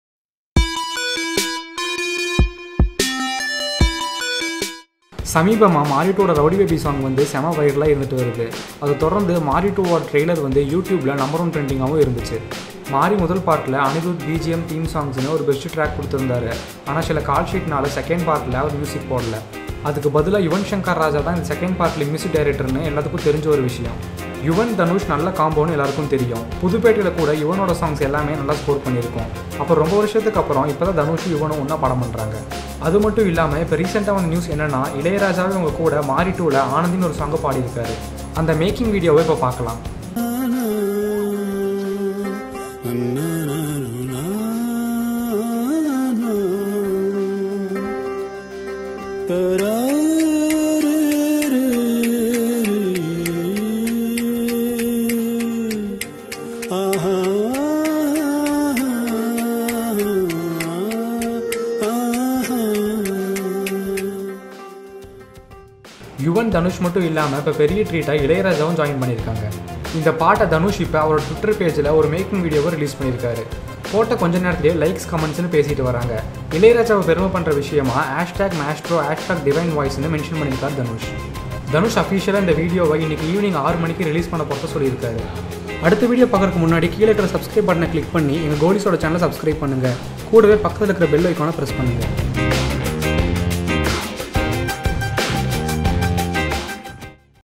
சற்றுமுன் இளையராஜாவுடன் தனுஷ் செய்ததை பாருங்க! | Tamil Cinema | Kollywood | Latest News
See what ilayaraja did with Dhanush!

சற்றுமுன் இளையராஜாவுடன் தனுஷ் செய்ததை பாருங்க!

Famous singer Ilayaraja & South Indian actor Dhanush plays music. In this video we are going to see what ilayaraja did with dhanush.